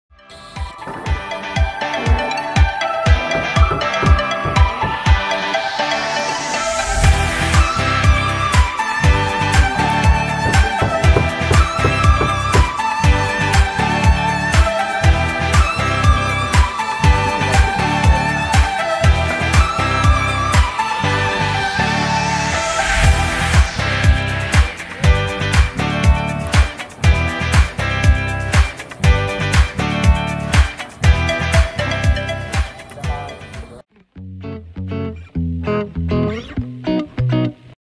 Canon SX420 IS Super Zoom!
Tested on 500 meters distance. Capturing the iconic NONAS,  NOAC, NSCA, CPSU gate!